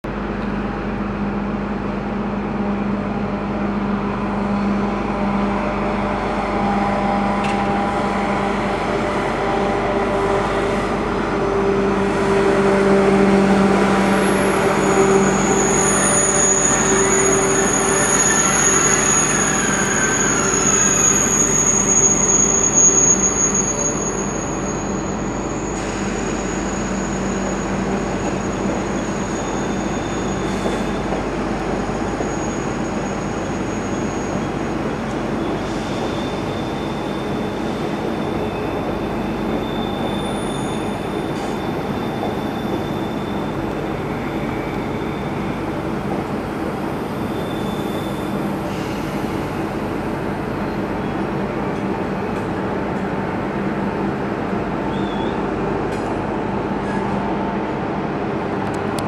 Eurostar class 373 arriving into London St Pancras International.
Eurostar class 373 arriving into London St Pancras International railway station.